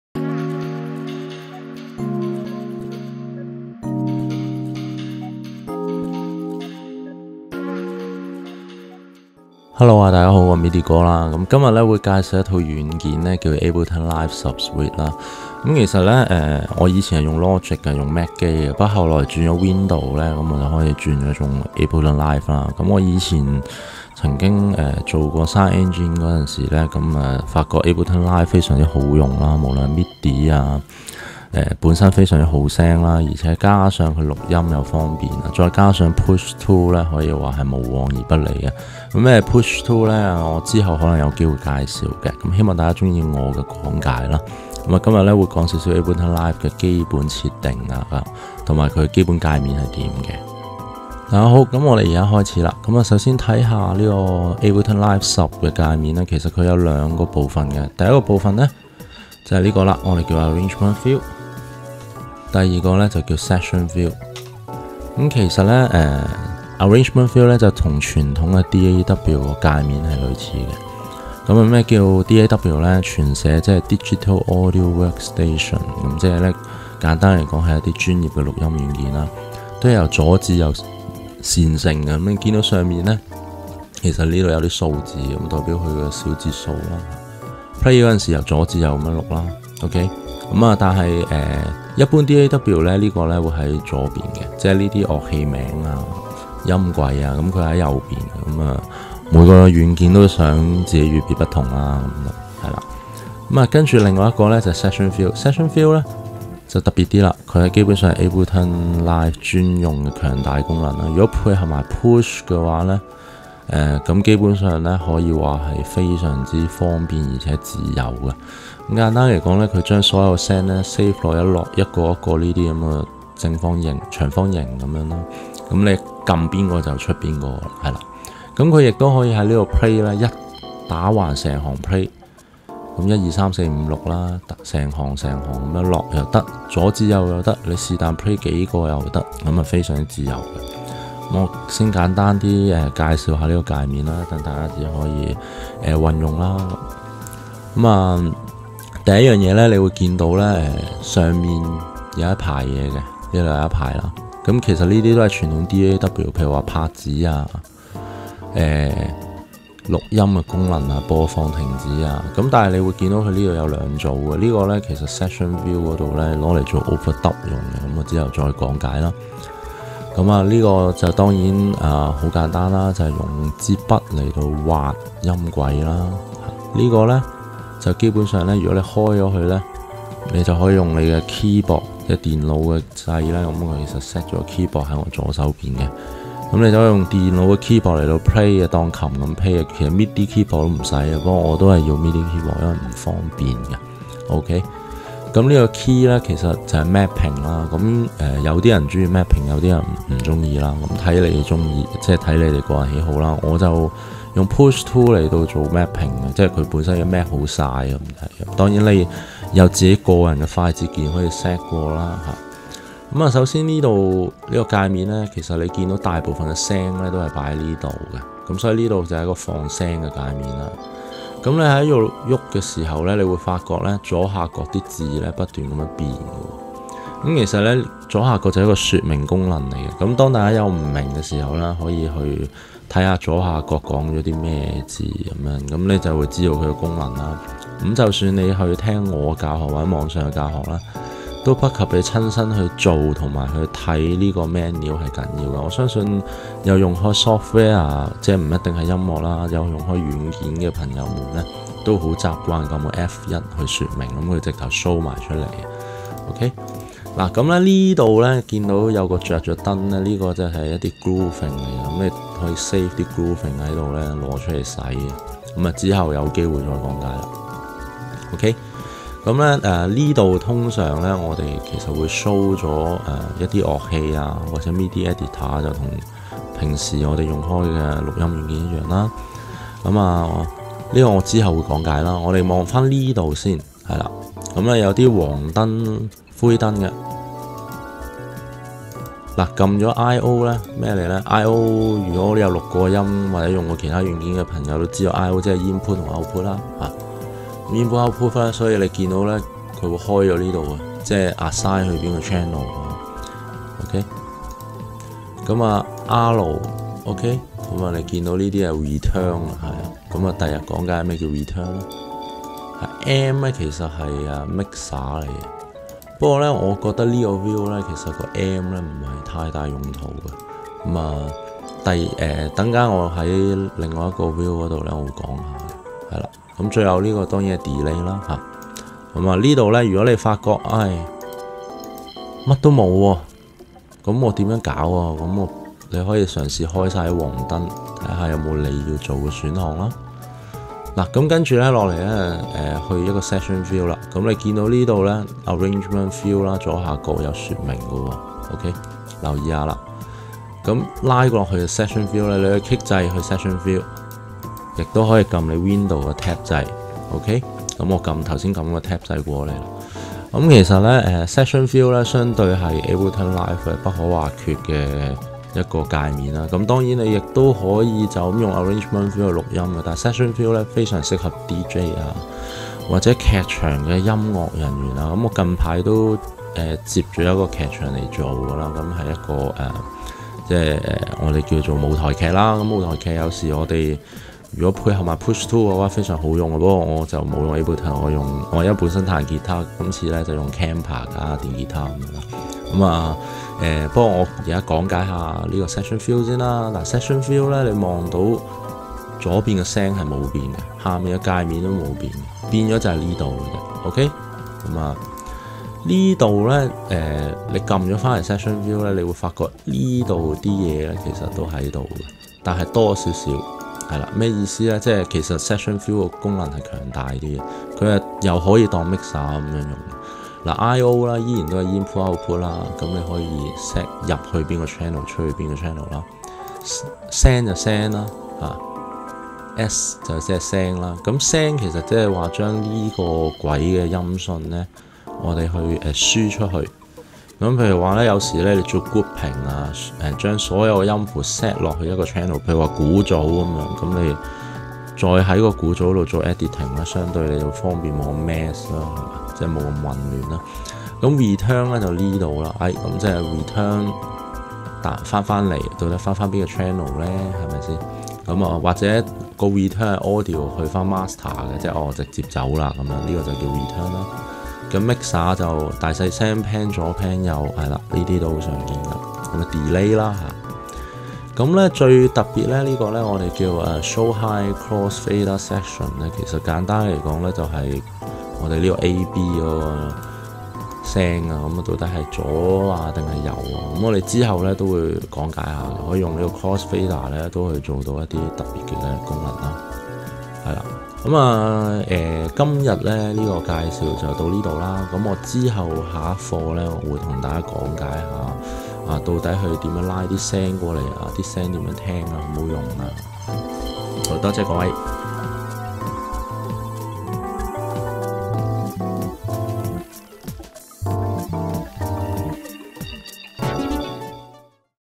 ㊙️Ableton Live 10/11教學第一集👩‍🏫👨‍🏫 - Midi歌既Ableton介面教學,咩叫DAW?(廣東話 粵語)
近來出左新既Ableton Live 11,有見及此出返Ableton教學,講解咩叫DAW, Arrangment view,同埋Session  View,之後會再update,感謝各位